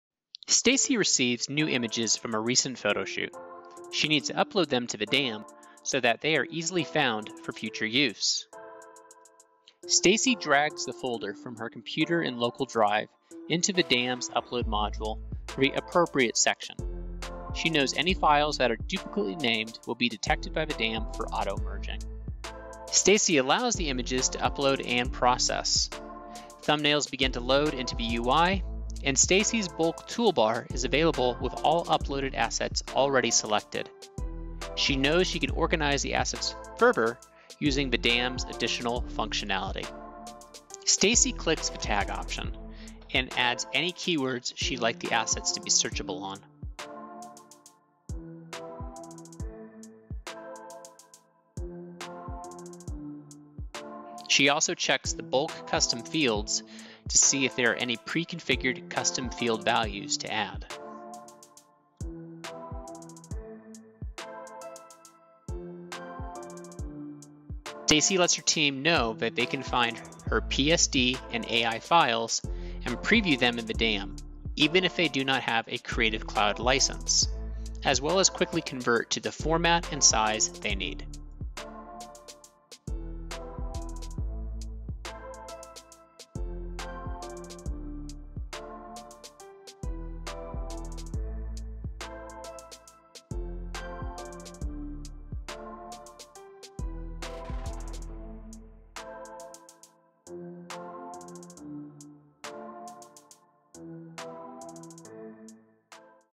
Cloudinary Digital Asset Management Scenario - Uploading Content to the DAM
The following video and associated material shows how to achieve a common digital asset management (DAM) scenario - uploading content - using Cloudinary's DAM solution.